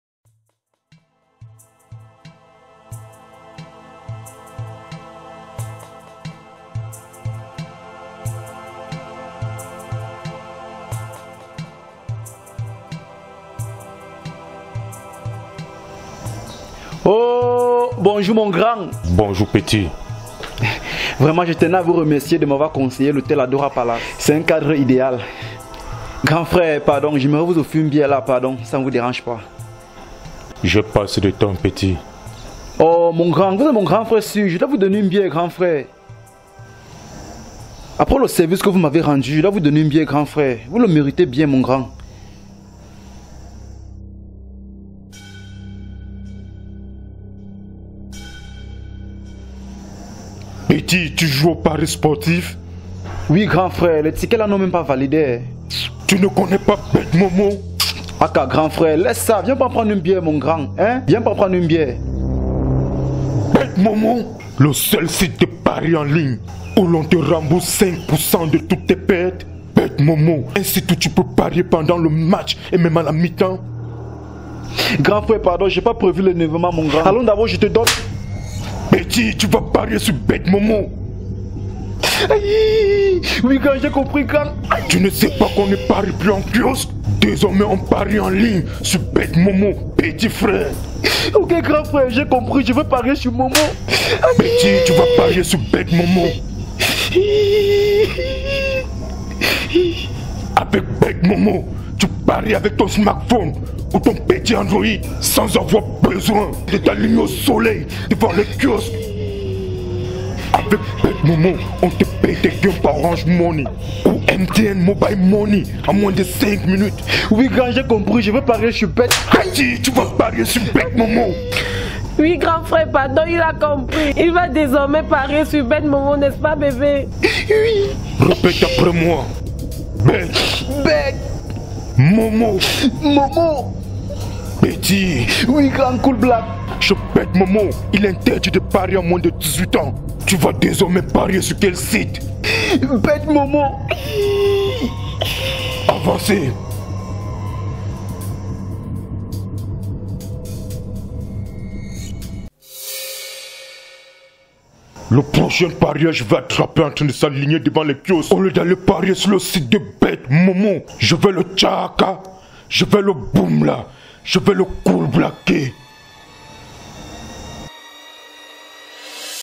PETIT !!! tu vas parier sur BETMOMO
apres ADORA PALACE regardez le deuxieme volet des aventures de EMPEROR , BEYONSSE et COOLBLACK
apres avoir ete force de dormir a ADORA PALACE le petit EMPEROR veut remercier le grand COOLBLACK de lui avoir conseille cet hotel,malheureusement pour lui COOL BLACK va decouvrir que le petit a besoin aussi d'un autre conseil tres important
la suite en video